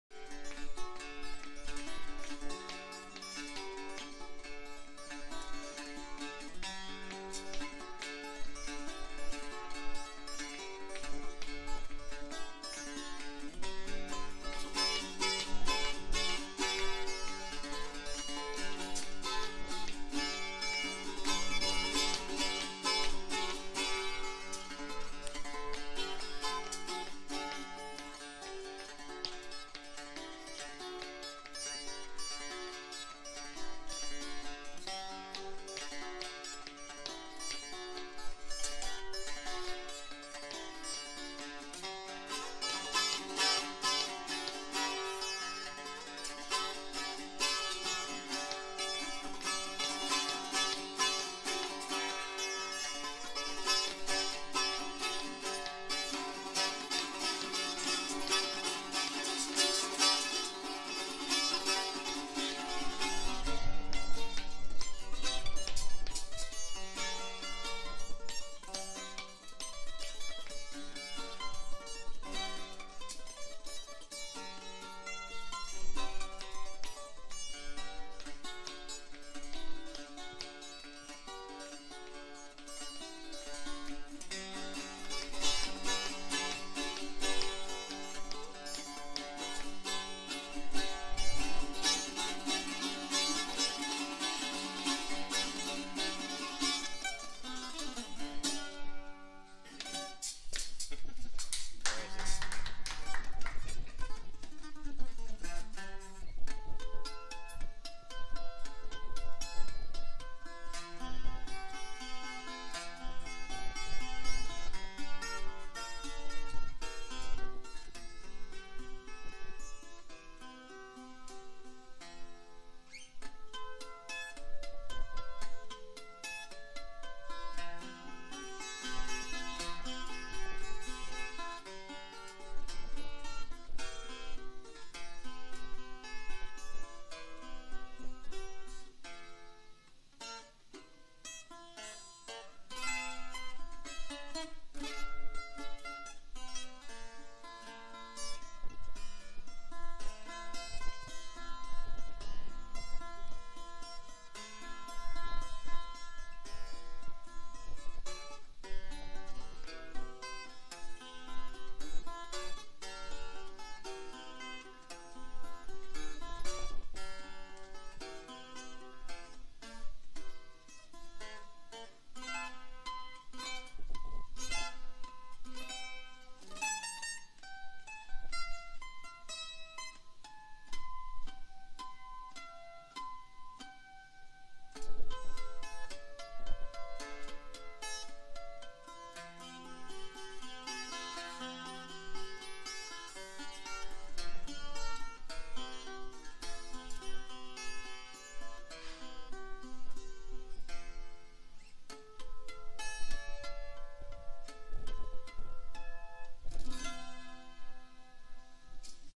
Veillette Gryphon jam at Maken Music with Ron Steta
Ron Steta stopped by the Make'n Music and picked up a Veillette Gryphon we had on the wall and started jamming.  It sounded so good that a picked up a flip cam and started recorded him.  Sorry for the low volume, the camera was set to have low input gain from a previous recording.